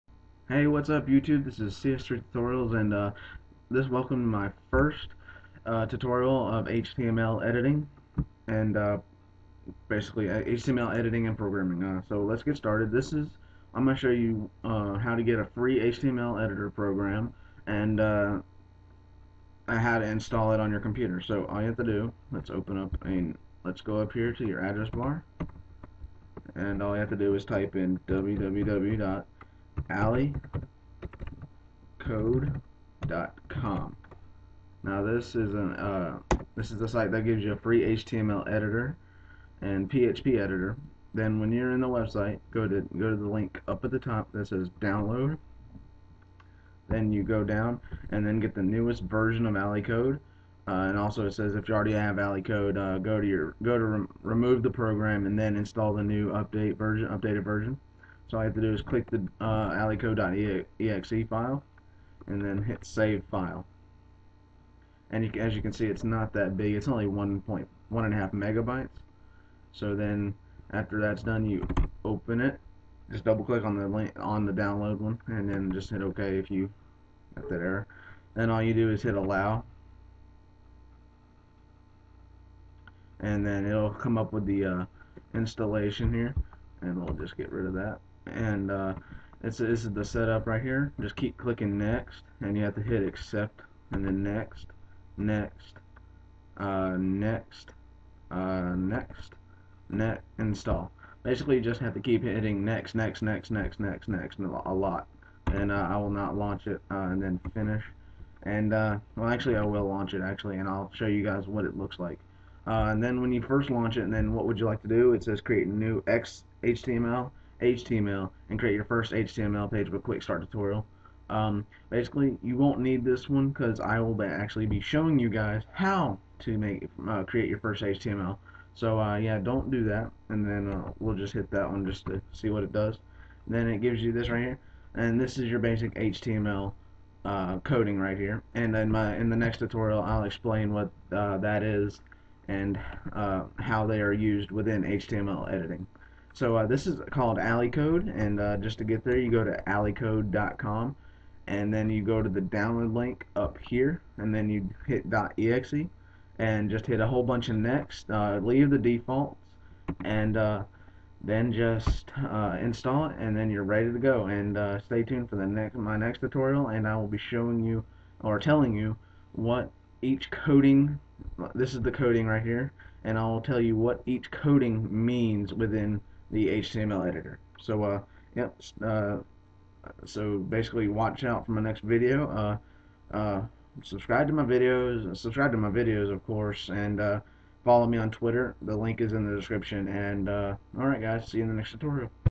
HTML Editing & Programming Tutorial -1- Installing AlleyCode
Hey guys this is how to download and install AlleyCode HTML Editor.